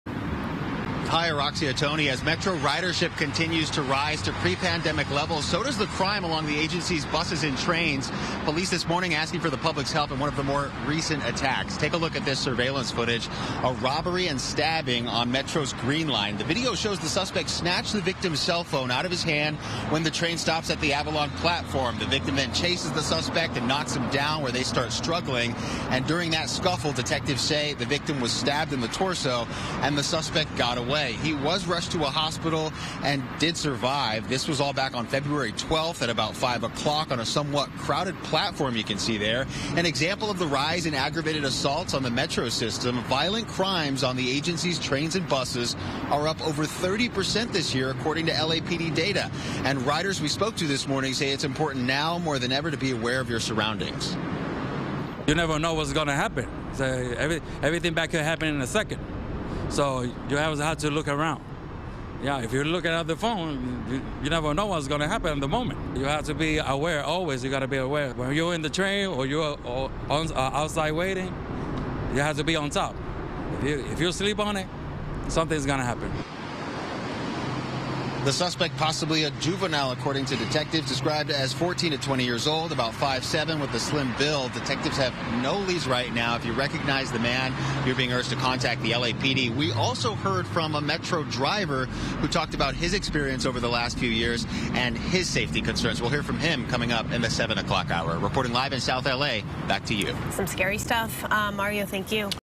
Police looking for suspect who stabbed man near South LA Metro train
Police are looking for a suspect who stabbed a man near the Avalon metro station while trying to steal his cell phone back in February.